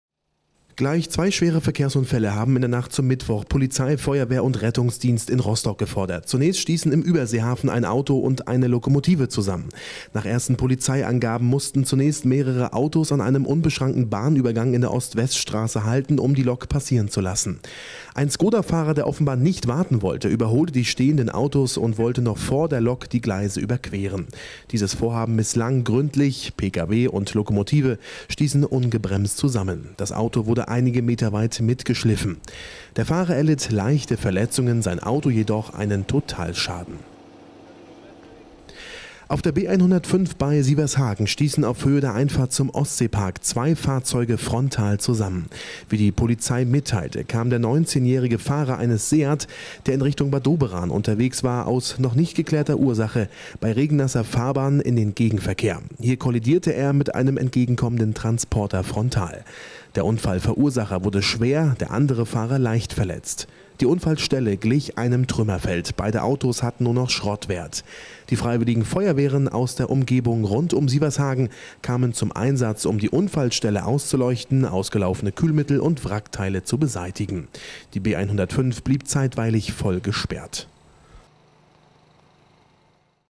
22.10.2014: Zwei schwere Unfälle beschäftigen Feuerwehr und Polizei in Rostock
Rostock-Stadtmitte - Gleich zwei schwere Unfälle haben in der Nacht zum Mittwoch Polizei, Feuerwehr und Rettungsdienst in Rostock gefordert. Zunächst stießen im Überseehafen ein Auto und eine Lokomotive zusammen. Nach ersten Polizeiangaben mussten zunächst mehrere Autos an einem unbeschrankten Bahnübergang in der Ost-West-Straße halten, um die Lok passieren zu lassen. Ein Skoda-Fahrer, der offenbar nicht warten wollte, überholte die stehenden Autos und wollte noch vor der Lok die Gleise überqueren. Dieses Vorhaben misslang gründlich. Pkw und Lokomotive stießen ungebremst zusammen. Das Auto wurde einige Meter weit mitgeschliffen. Der Fahrer erlitt leichte Verletzungen. Sein Auto jedoch einen Totalschaden.
Auf der B 105 bei Sievershagen stießen auf Höhe der Einfahrt zum Ostseepark zwei Fahrzeuge frontal zusammen. Wie die Polizei mitteilte, kam der 19-jährige Fahrer eines Seat, der in Richtung Bad Doberan unterwegs war, aus noch nicht geklärter Ursache bei regennasser Fahrbahn in den Gegenverkehr. Hier kollidierte er mit einem entgegenkommenden Transporter frontal. Der Unfallverursacher wurde schwer, der andere Fahrer leicht verletzt. Die Unfallstelle glich einem Trümmerfeld, beide Autos hatten nur noch Schrottwert. Die Freiwilligen Feuerwehren aus der Umgebung rund um Sievershagen kamen zum Einsatz, um die Unfallstelle auszuleuchten, ausgelaufene Kühlmittel und Wrackteile zu beseitigen. Die B 105 blieb zeitweilig voll gesperrt.